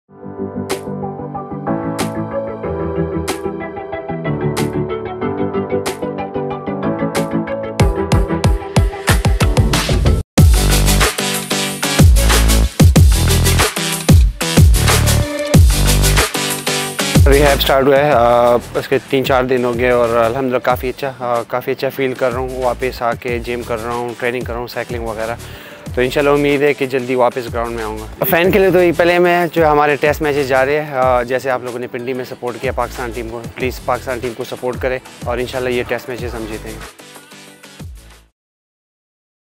Shaheen Afridi Gives Update About His Rehabilitation | PCB | MA2L
On the road to recovery - Shaheen Afridi continues his rehabilitation at NHPC, Lahore.